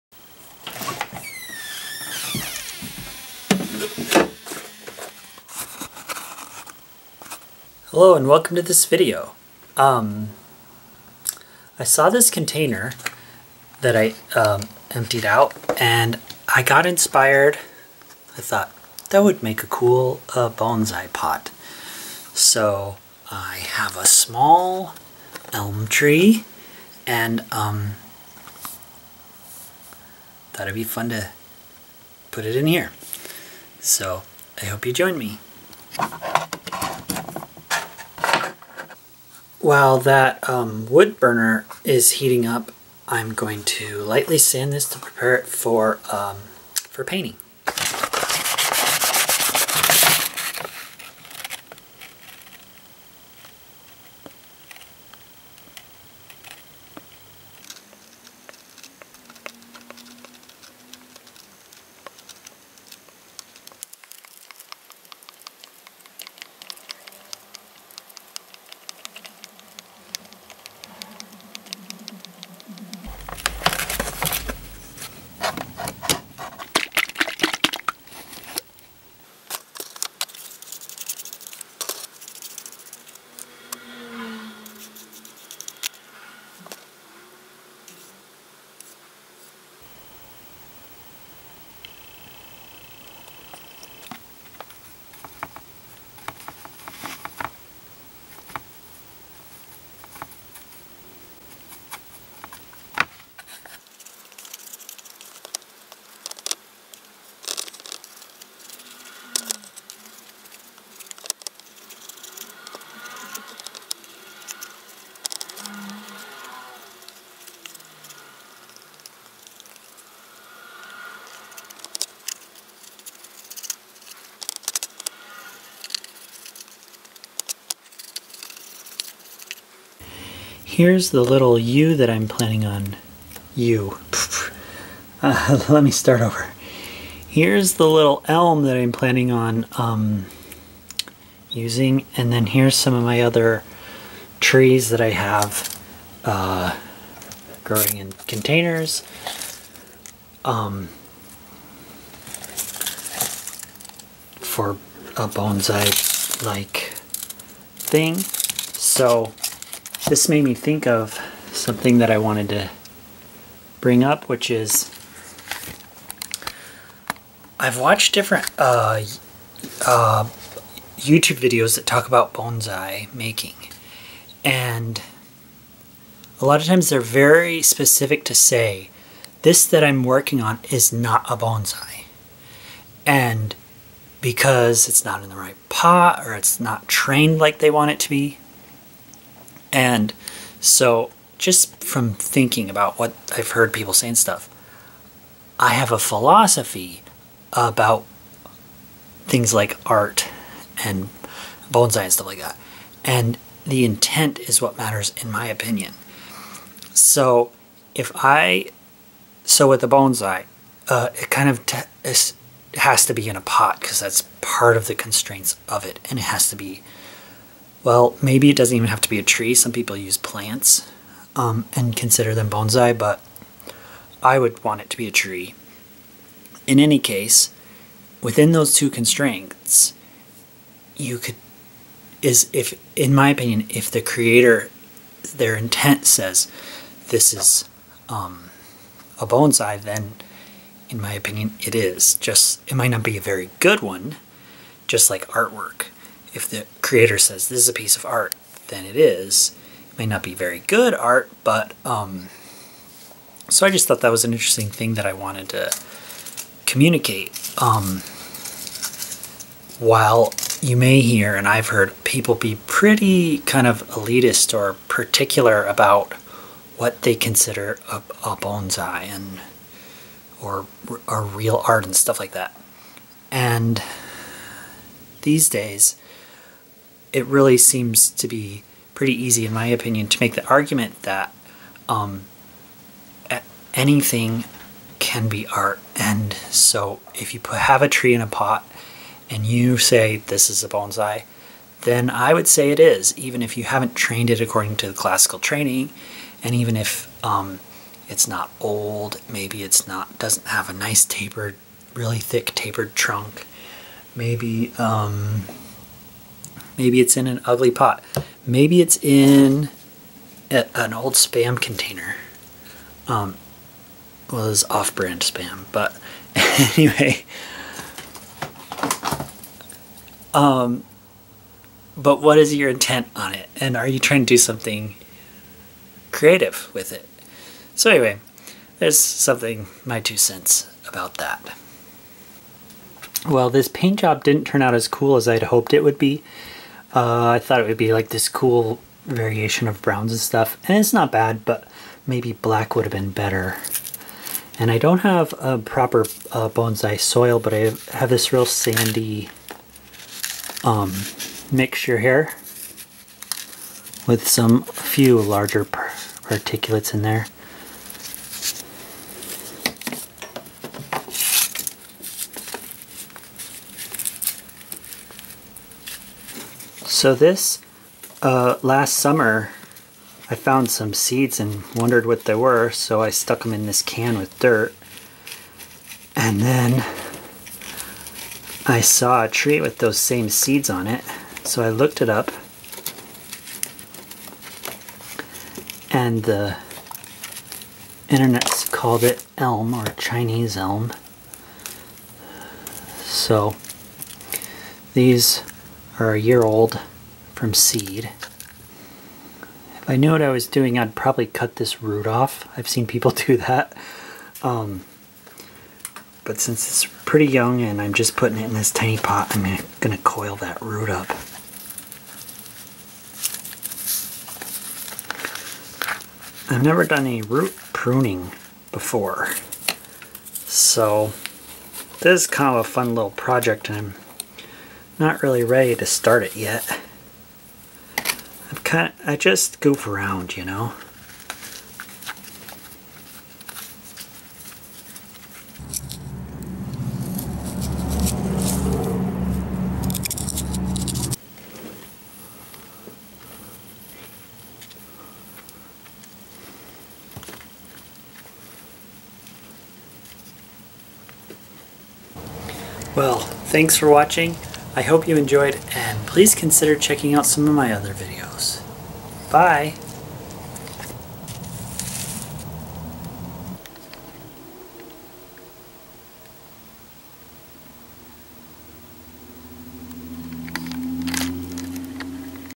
DIY Mini Bonsai Pot
In this video I pot a young elm sapling in a DIY bonsai pot. I also discuss the idea of what makes a potted tree a bonsai. I hope you enjoy.